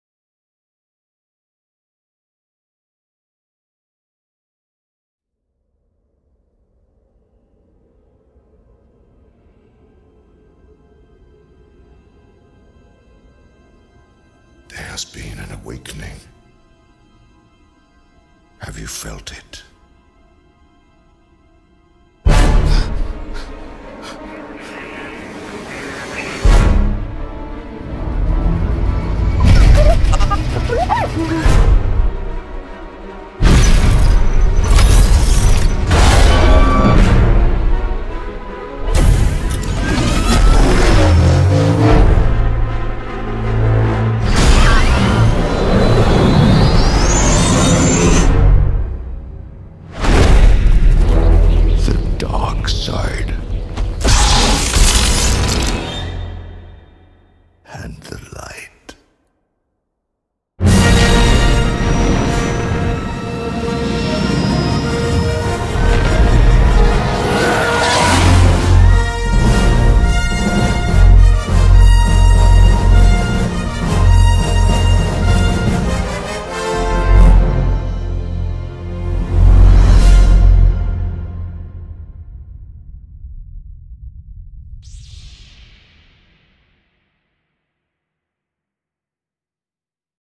Star Wars Episode VII
The beginning of a new Star Wars trilogy will take place 30 years after Episode VI: Return of the Jedi. The new films will feature "a trio of new young leads along with some very familiar faces" (per StarWars.com).